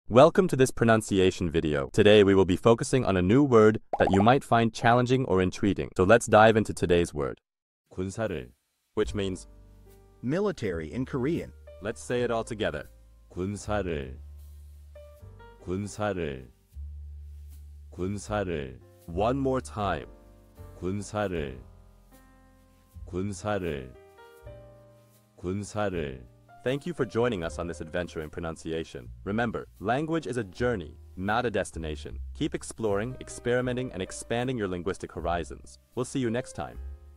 How to pronounce gunsareul | 군사를 (Military in Korean)
Master the Pronunciation of 'gunsareul | 군사를' - which means : Military in Korean 📝 with @PronunciationKorean 🎓🗣️ - [Your Guide to Korean]

Learn to pronounce 'gunsareul' perfectly in Korean with our easy and engaging tutorial by @PronunciationKorean! 📖📽️ In this comprehensive video, we provide you with native speaker examples 🗣️👂 and phonetic breakdowns 🎵 to ensure you get the pronunciation just right.